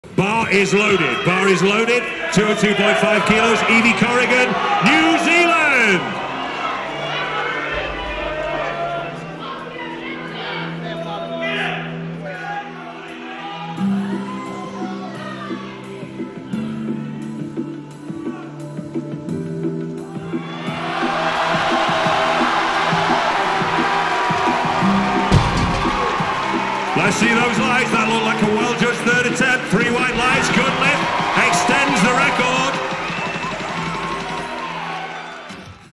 Sheffield 2023 - Evie Corrigan World Record 202.5kg Deadlift
A new world record for Evie Corrigan of New Zealand with a 202.5kg deadlift in the 52kg class at the Sheffield Powerlifting Championships in the United Kingdom.